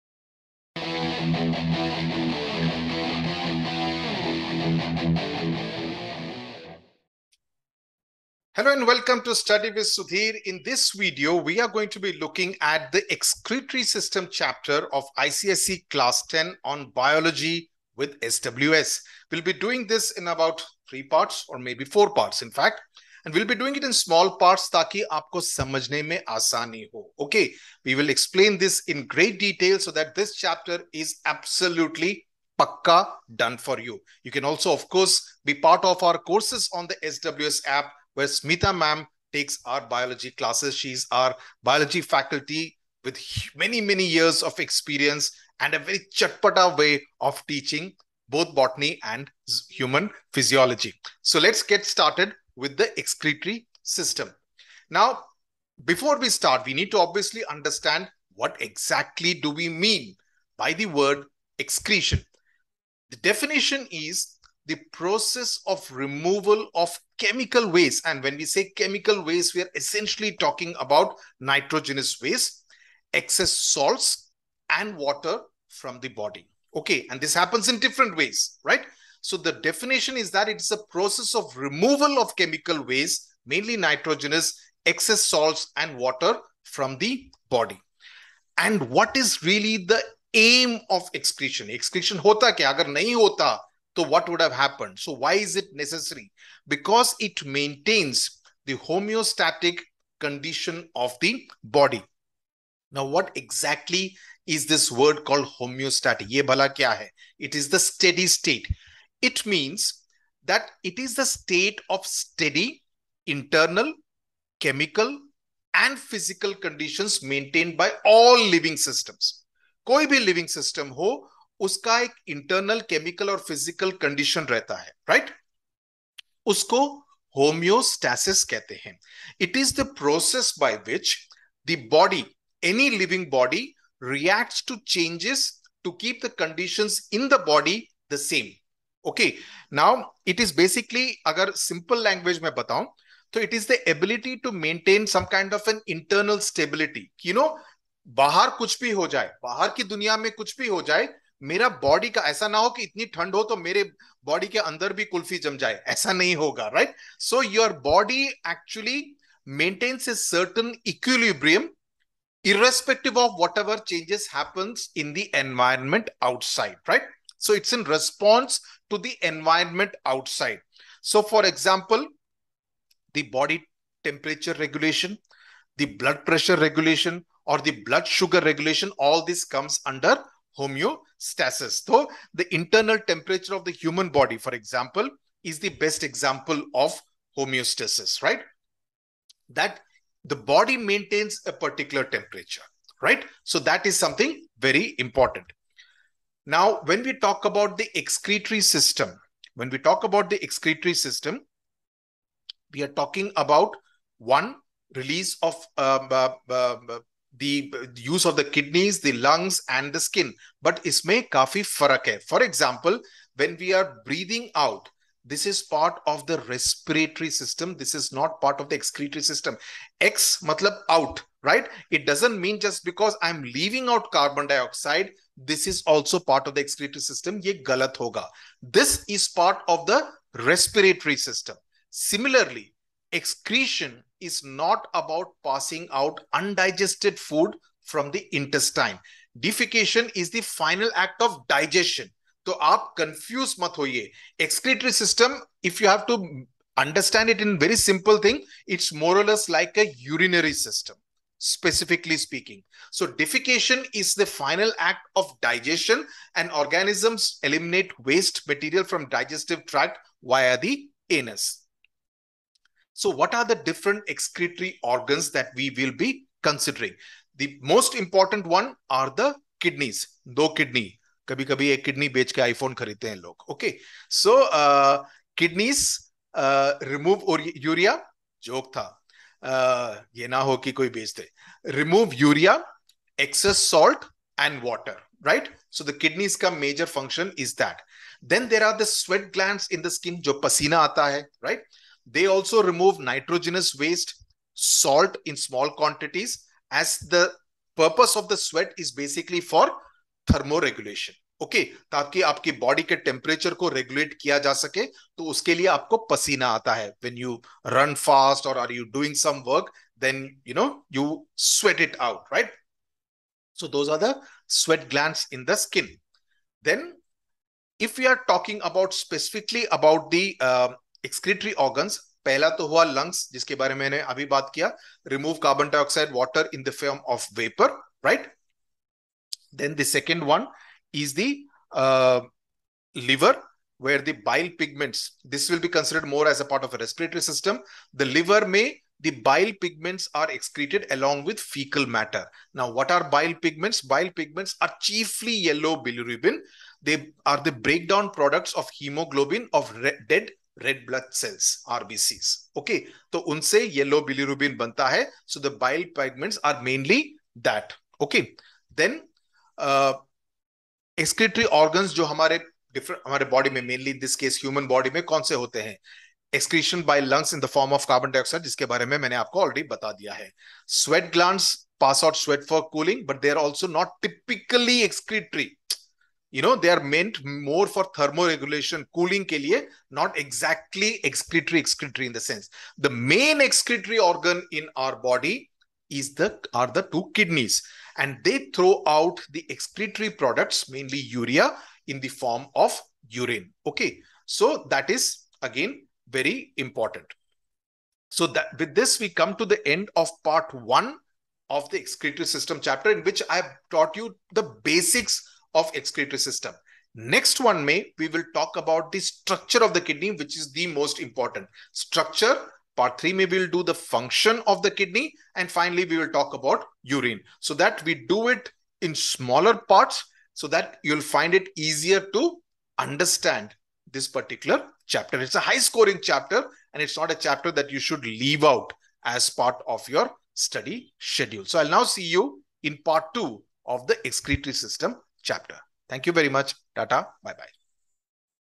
Excretory System (Part 1) | Understanding Excretion | Biology with Sudhir Sir | ICSE 10 | SWS
In this video, I teach the Excretory System for Biology students of Class 10 ICSE

                        Telegram : @Study with Sudhir